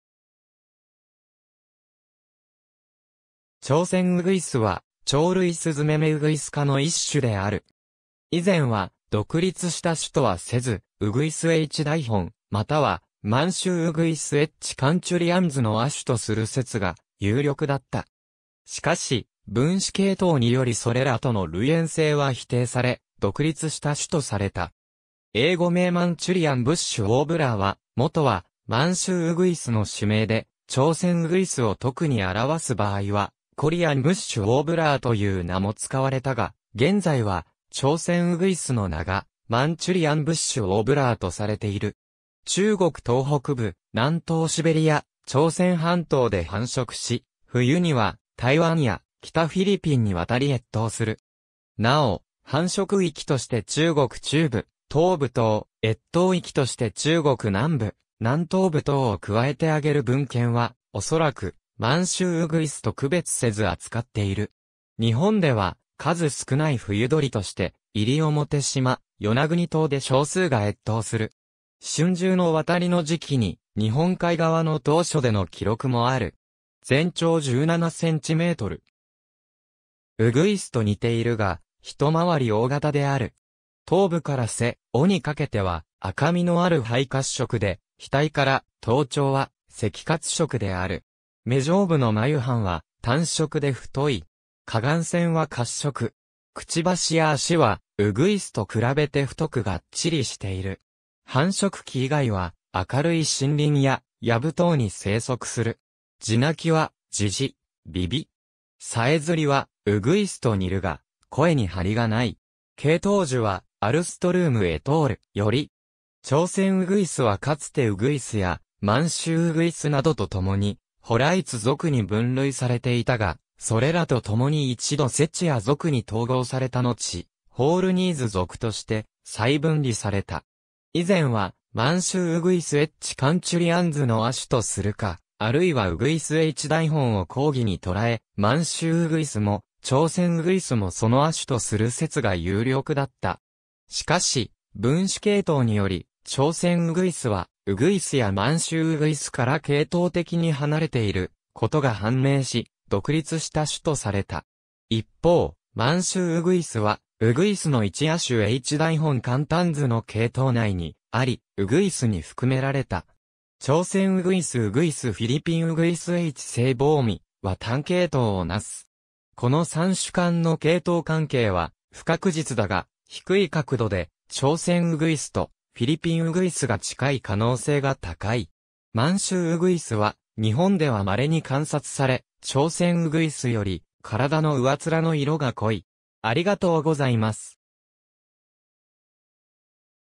チョウセンウグイス
チョウセンウグイス（朝鮮鶯、学名: Horornis borealis）は、鳥類スズメ目ウグイス科の1種である。
以前は独立した種とはせず、ウグイス H. diphone またはマンシュウウグイス H. canturians の亜種とする説が有力だった。
しかし分子系統によりそれらとの類縁性は否定され、独立した種とされた。
英語名 Manchurian Bush Warbler は元はマンシュウウグイス（チョウセンウグイスを亜種として含みうる）の種名で、チョウセンウグイスを特に表す場合は Korean Bush Warbler という名も使われたが、現在はチョウセンウグイスの名が Manchurian Bush Warbler とされている。
中国東北部、南東シベリア、朝鮮半島で繁殖し、冬には台湾や北フィリピンに渡り越冬する。
なお、繁殖域として中国中部・東部等、越冬域として中国南部・南東部等を加えて挙げる文献は、おそらくマンシュウウグイスと区別せず扱っている。
日本では数少ない冬鳥として、西表島、与那国島で少数が越冬する。
春秋の渡りの時期に、日本海側の島嶼での記録もある。
全長17cm。
ウグイスと似ているが、一回り大型である。
頭部から背、尾にかけては赤みのある灰褐色で、額から頭頂は赤褐色である。
眼上部の眉斑は淡色で太い。
過眼線は褐色。
嘴や足は、ウグイスと比べて太くがっちりしている。
繁殖期以外は、明るい森林や藪等に生息する。
地鳴きは、「ジジッ」「ビビッ」。
さえずりはウグイスと似るが、声に張りがない。
系統樹は Alström et al. (2011)より。
チョウセンウグイスはかつてウグイスやマンシュウウグイスなどと共に Horeites 属に分類されていたが、それらと共に一度 Cettia 属に統合されたのち、 Horornis 属として再分離された。
以前は、マンシュウウグイス H. canturians の亜種とするか、あるいはウグイス H. diphone を広義にとらえマンシュウウグイスもチョウセンウグイスもその亜種とする説が有力だった。
しかし分子系統により、チョウセンウグイスはウグイスやマンシュウウグイスから系統的に離れていることが判明し、独立した種とされた。
一方、マンシュウウグイスはウグイスの1亜種 H. diphone cantans の系統内にあり、ウグイスに含められた。
チョウセンウグイス+ウグイス（マンシュウウグイスを含む）+フィリピンウグイス H. seebohmi は単系統をなす。
この3種間の系統関係は不確実だが、低い確度で、チョウセンウグイスとフィリピンウグイスが近い可能性が高い。
マンシュウウグイスは日本ではまれに観察され、チョウセンウグイスより体の上面の色が濃い。